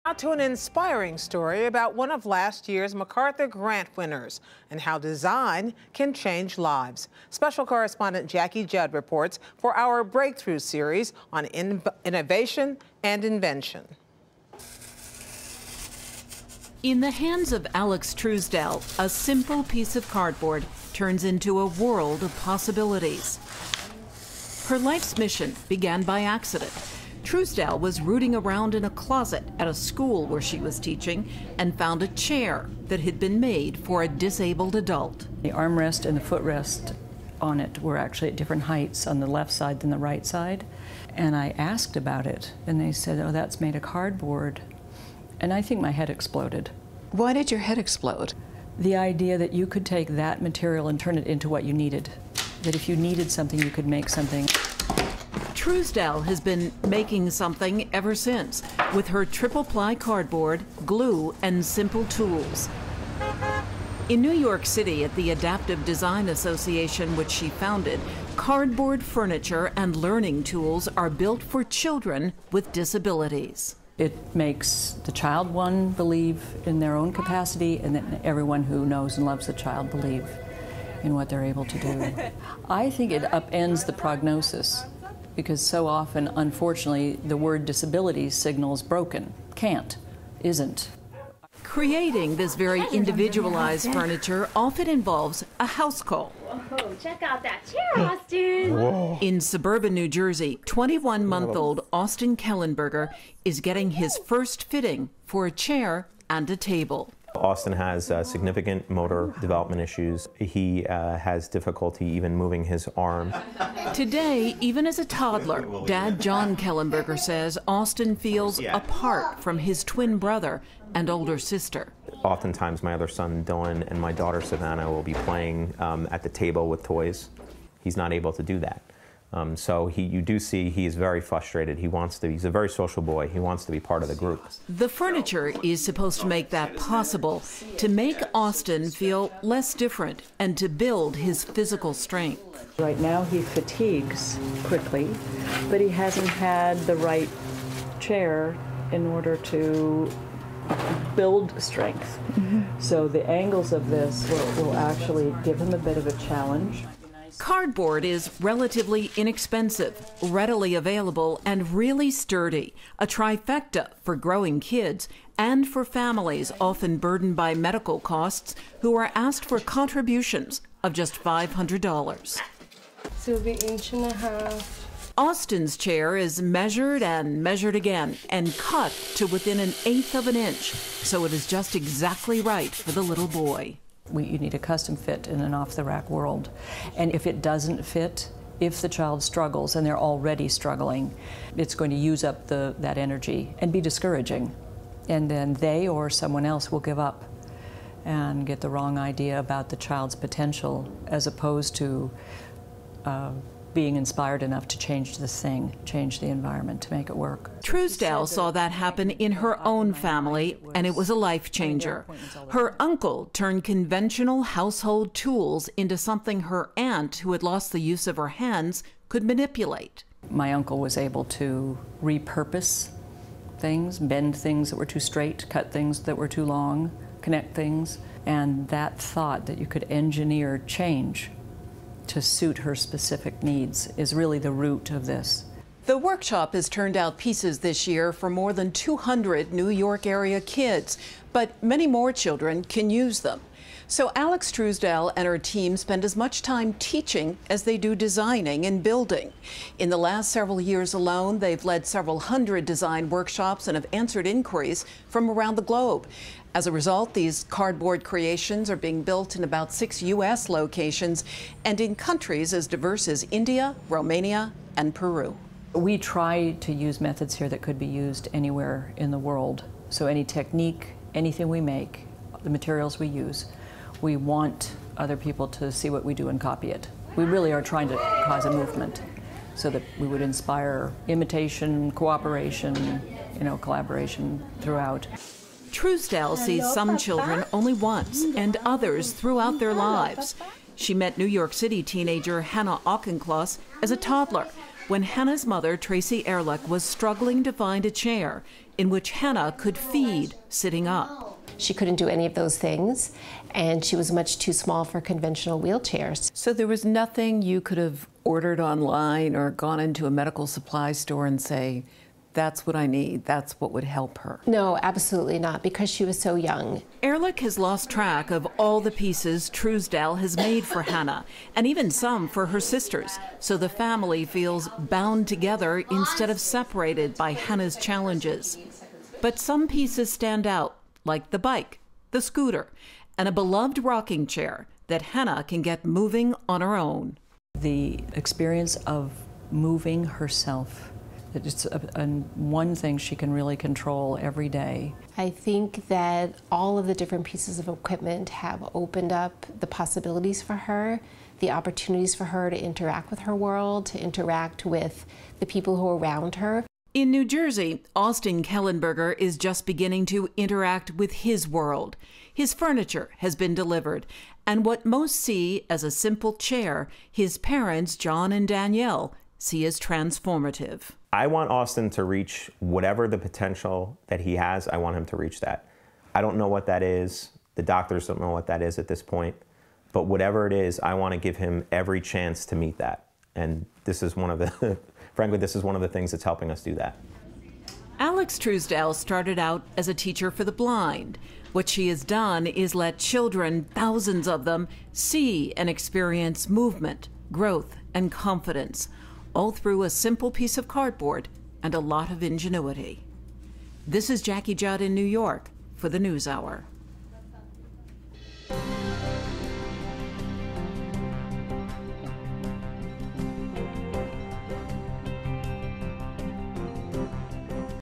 For children with disabilities, making the world a custom fit out of cardboard
MacArthur fellow Alex Truesdell sees a world of possibilities in a simple piece of cardboard. At her Adaptive Design Association, cardboard furniture and learning tools are built for children with disabilities to help them realize their potential. Special correspondent Jackie Judd reports.